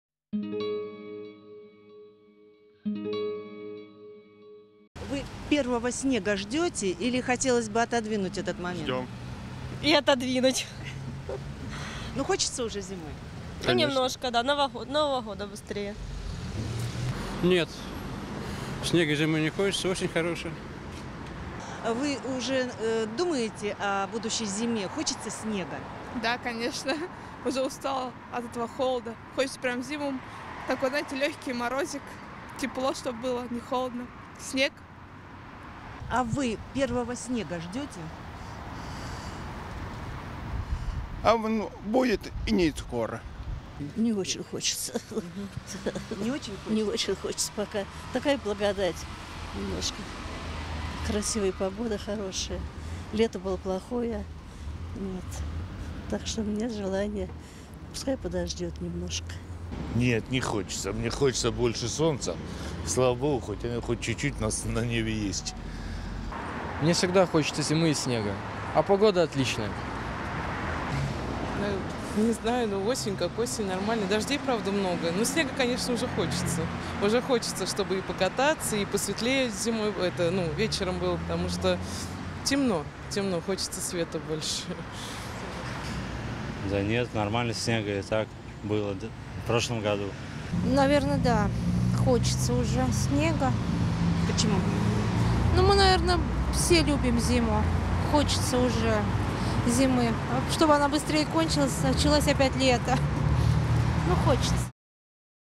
Ждёте ли первый снег?
9 октября 2017 | Сергиевопосадцы разделились на два лагеря. Одни ценят момент – золотую осень октября – и не хотят скоро с ней расставаться. Другие – по большей части, это молодёжь – ждут хорошего впереди, с наступлением зимы. Уже прозвучали ключевые слова «Новый год».